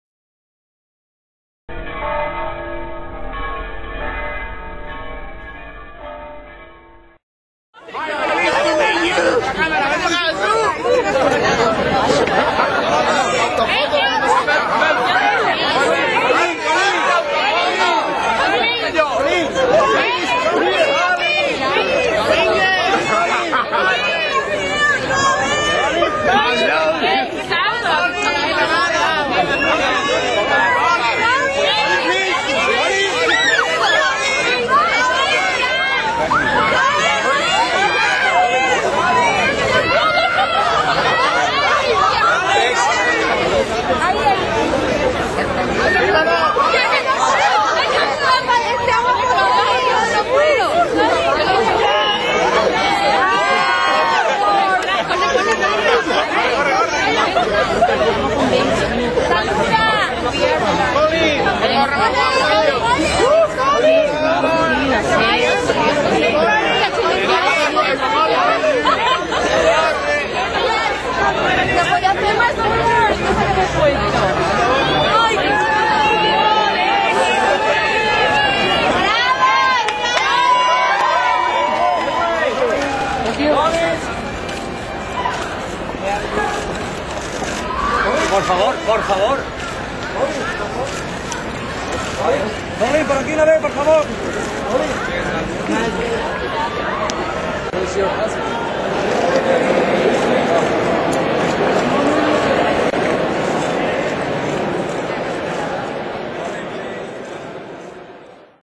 Colin Farrell en Sevilla
Entrada de Colin Farrell a la Gala del 6 de noviembre del Sevilla Festival de Cine 2009